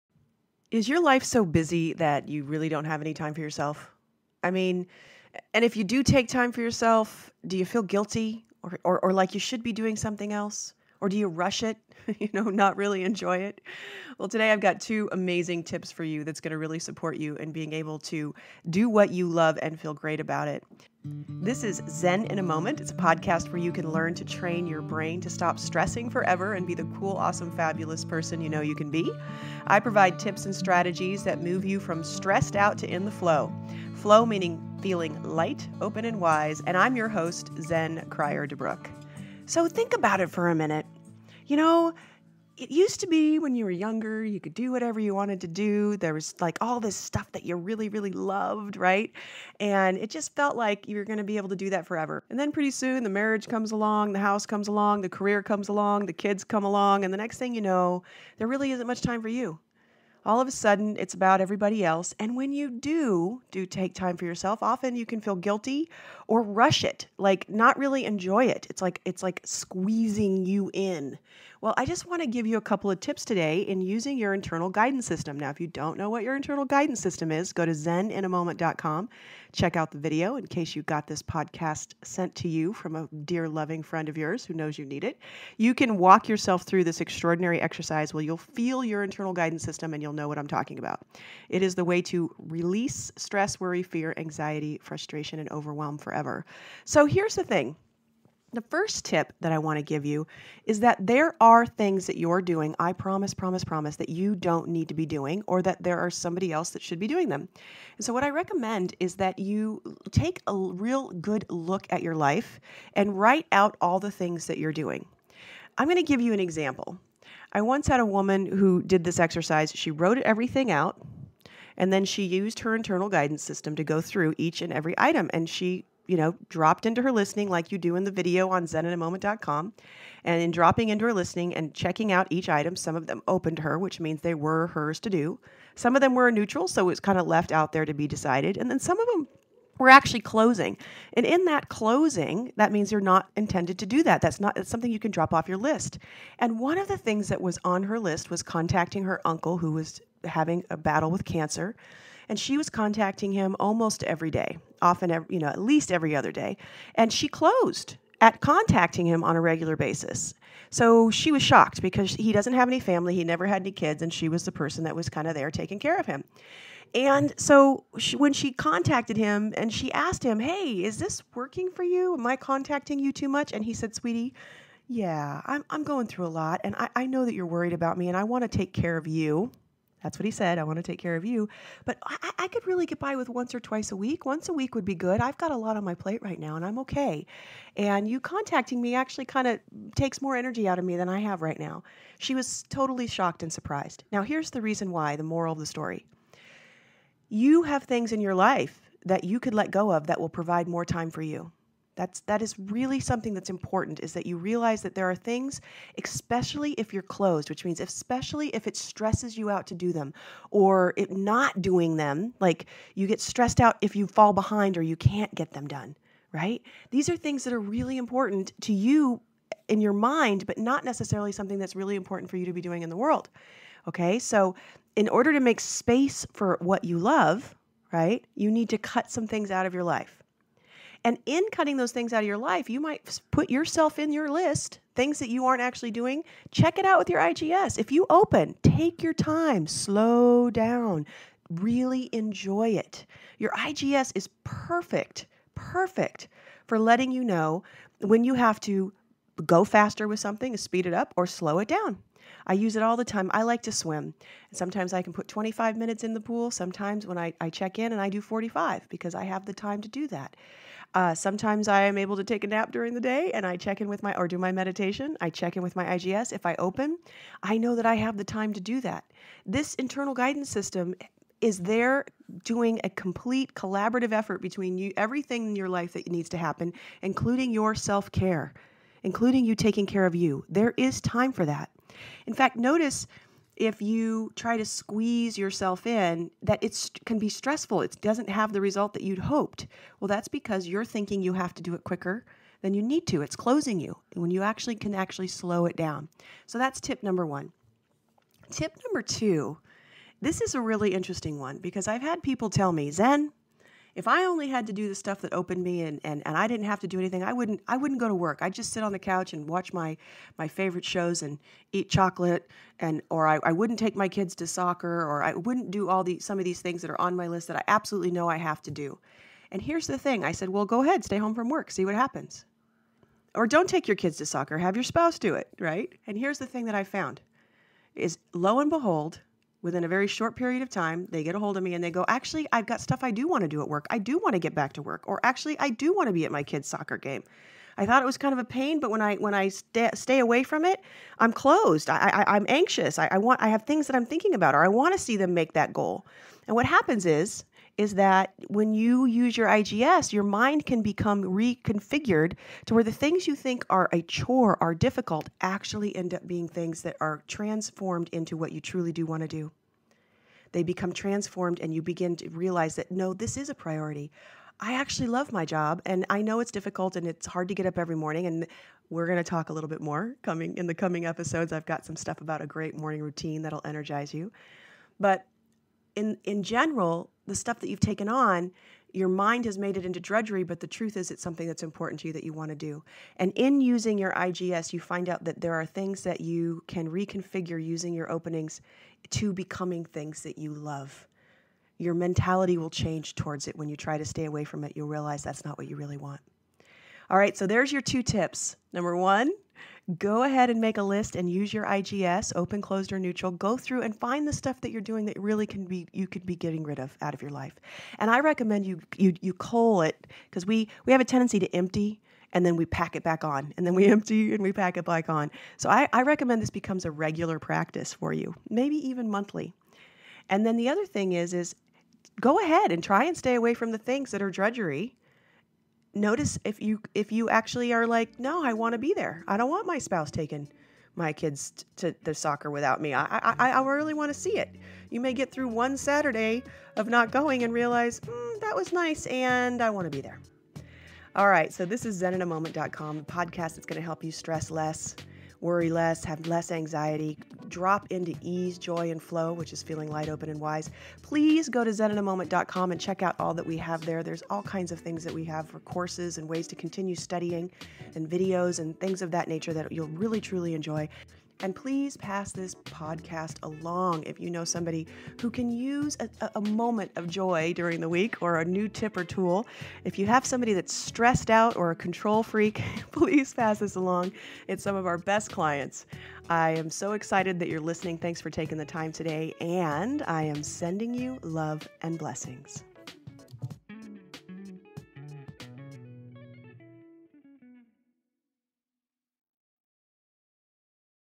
How To Make Time For The Things You Love
Is your life so busy that you really don’t have any time for yourself?

And if you do take time for yourself, do you feel guilty?

But taking care of YOU will help you stress less, worry less, and have less anxiety. Today I’m going to share two tips that will show you exactly how you can make space for what you love.